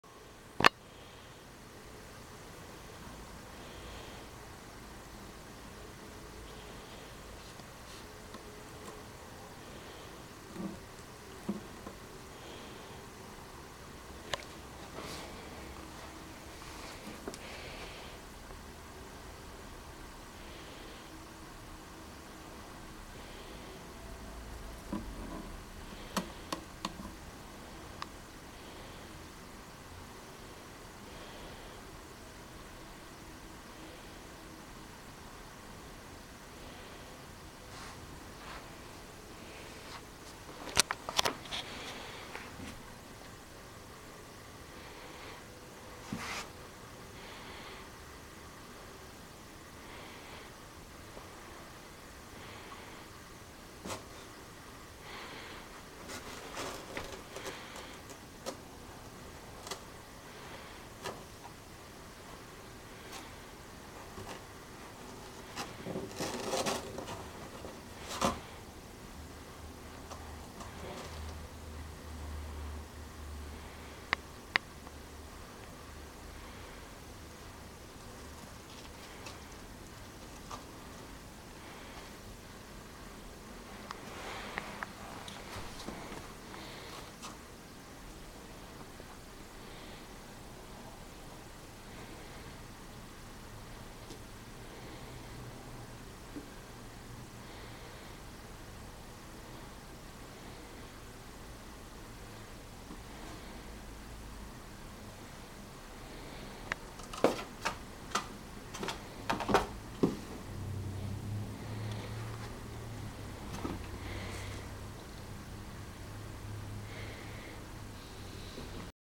Kitten In a Box
Orville, the kitten, is in a box. Cornelius wants to join him.